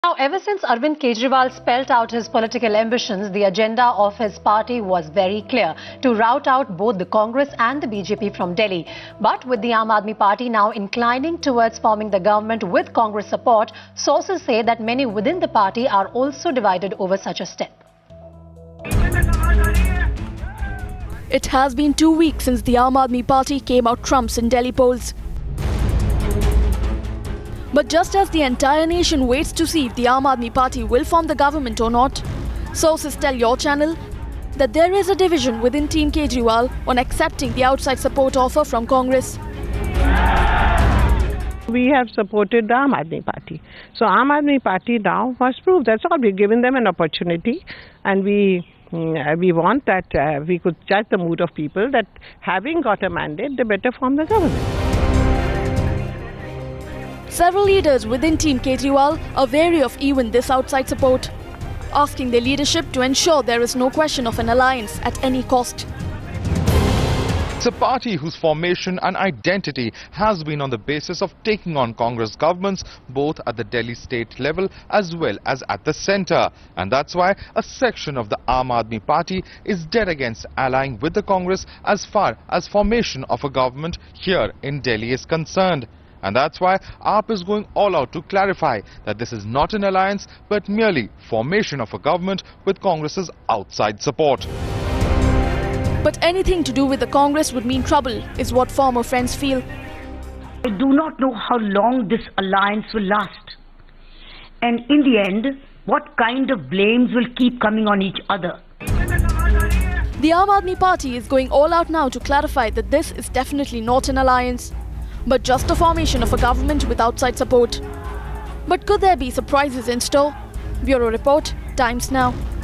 Divide within the AAP?
Ever since Arvind Kejriwal spelt out his political ambitions, the agenda of his party was very clear -- to rout out both the Congress and the BJP from Delhi. But with the Aam Aadmi Party now inclining towards forming the government with Congress support, sources say that many within the party are divided over such a step. Download the Times Now India's Election HQ app and get all the election info at one go. Social Media Links